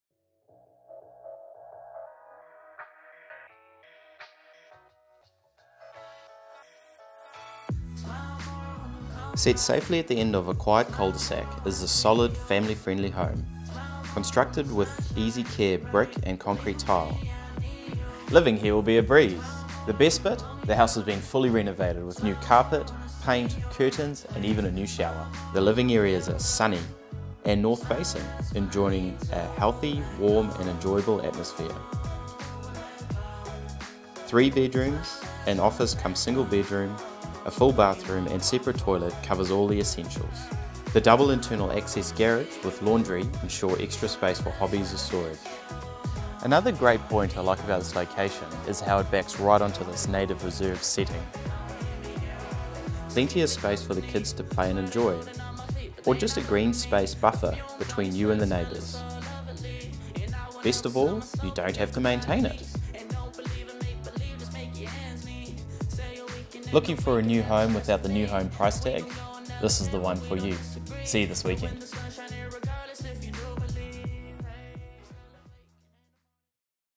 SOLD - 11 Sapling Drive, Pukekohe - Ian du Plessis and Elizabeth du Plessis
Sunny & Renovated with Reserve Setting

Come and enjoy this beautifully renovated home set safely at the end of a quiet cul-de-sac. Solid construction, brick and concrete tile, ensures low maintenance carefree living is this family ...

Property Marketed by: Ian du Plessis, Pukekohe and Elizabeth du Plessis, Pukekohe